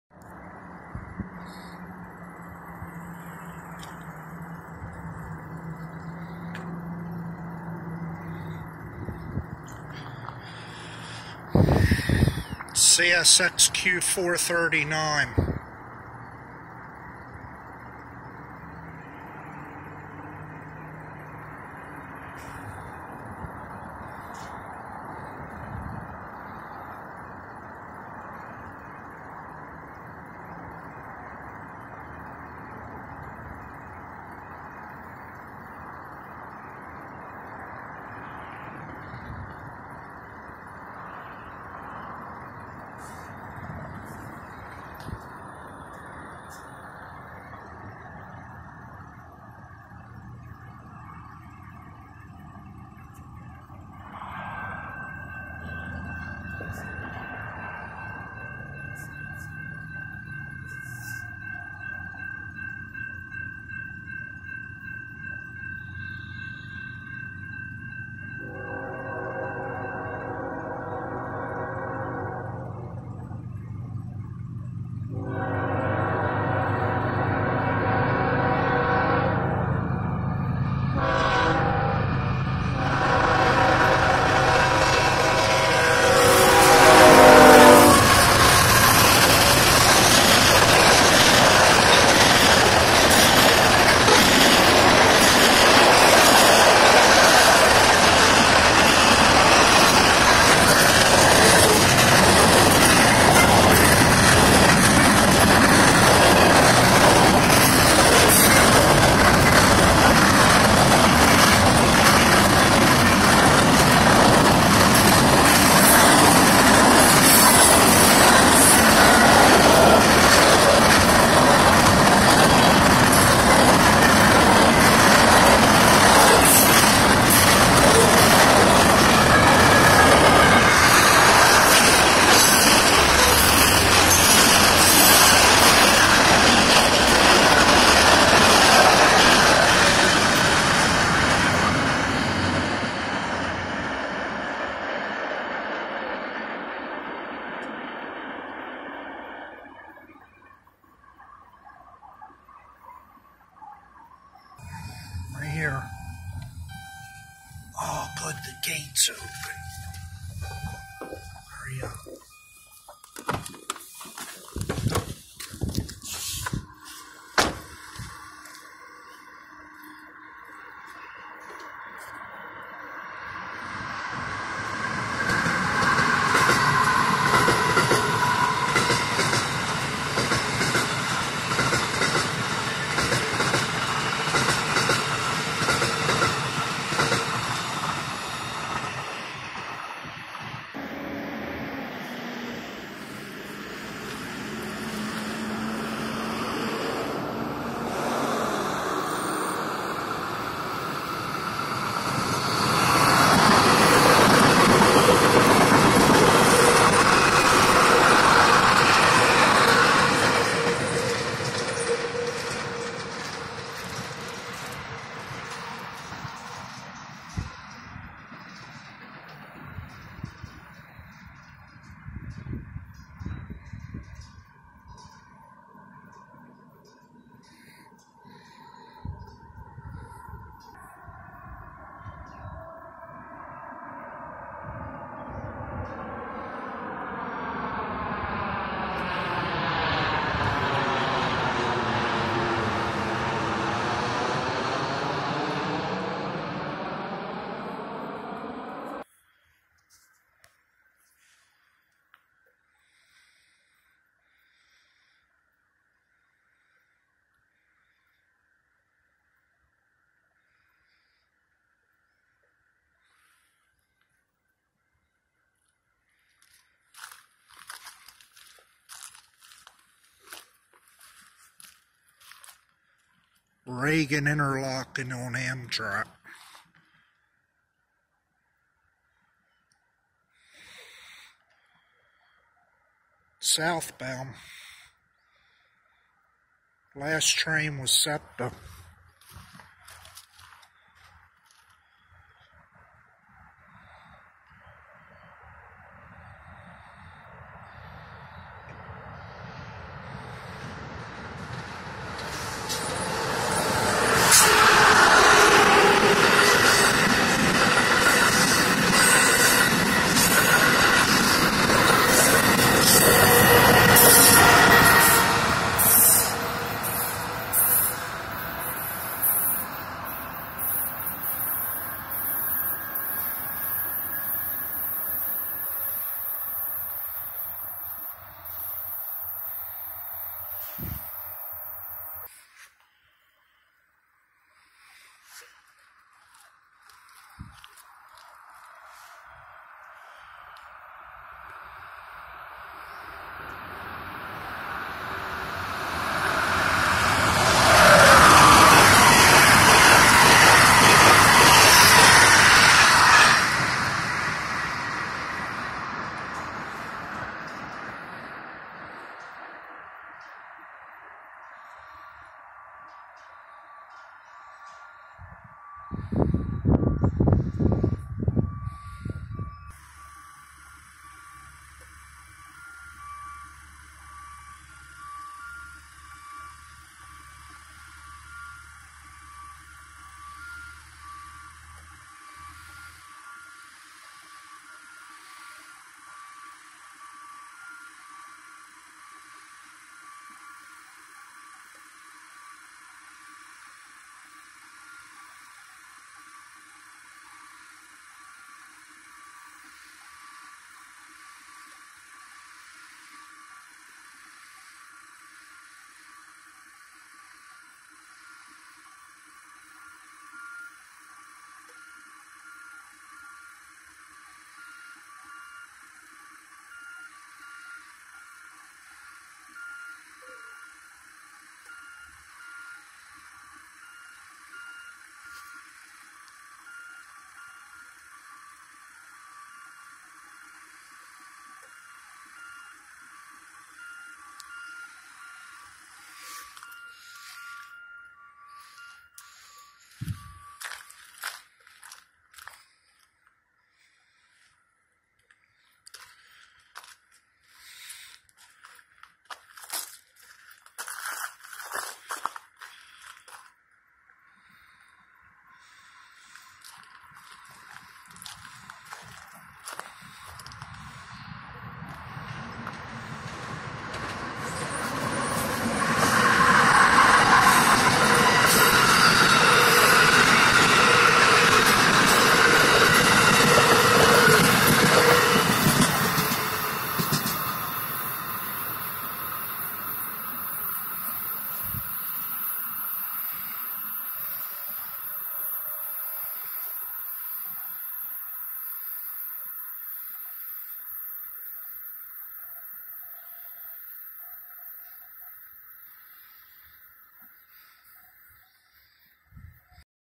CSX AND AMTRAK TRAINS
Here on Friday evening February 22nd 2019 I catch westbound train Q439 whizzing by Centerville road crossing on a clear signal on the main for them. After seeing this train we headed over to the Amtrak Northeast corridor to catch some pm rush hour trains. We caught a variety of trains before me and my ride decided to head home for the night. Enjoy